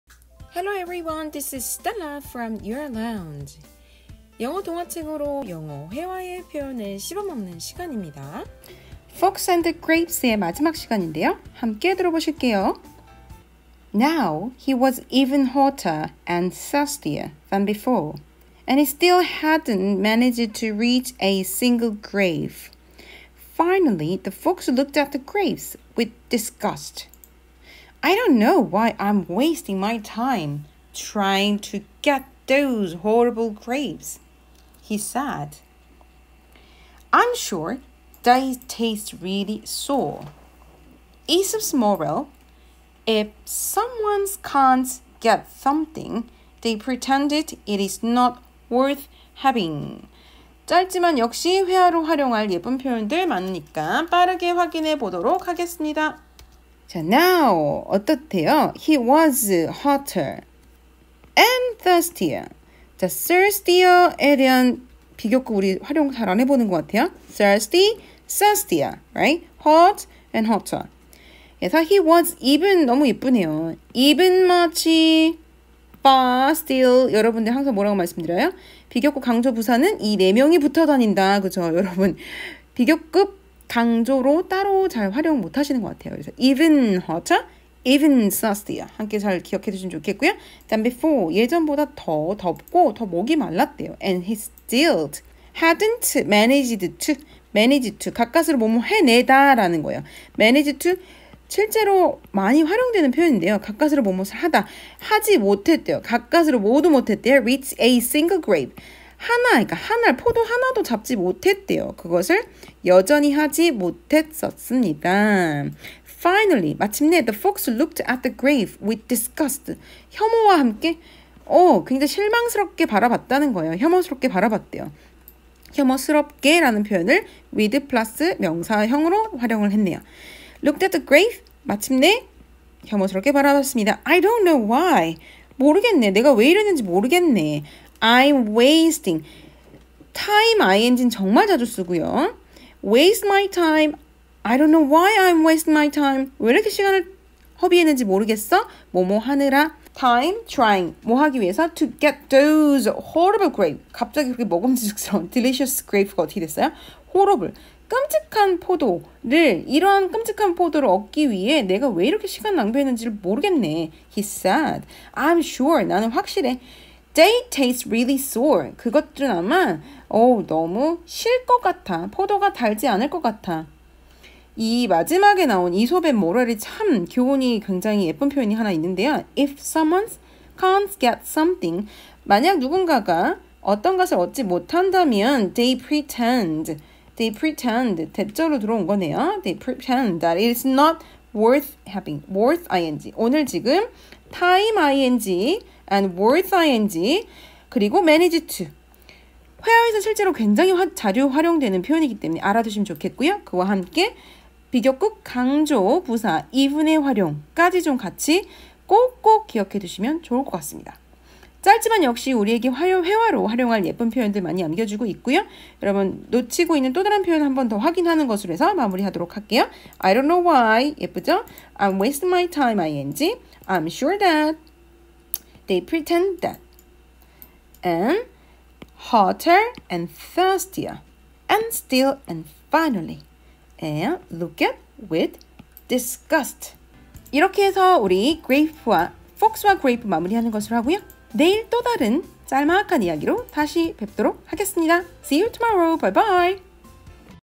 영어회화에 유용한 표현 | 꼭꼭 씹어먹기 | 귀로들으면 회화가 술술되는 영어동화습관
매일 아침 영어동화습관~!
준비되셨나요~!? :)

여러분들은 비교급 강조부사 even much far still을 얼마나 활용하시나요?
수업시간에 배워본 spend time ~ing는요?
worth ~ing도 모르는건 아닌데 자꾸 활용이 안되시죠?
그럼~ 오늘의 영상 반복해서 꼭꼭 씹어드세요 :)

그럼~ 영어회화에 이쁜 표현 마구마구 활용하실 수 있답니다~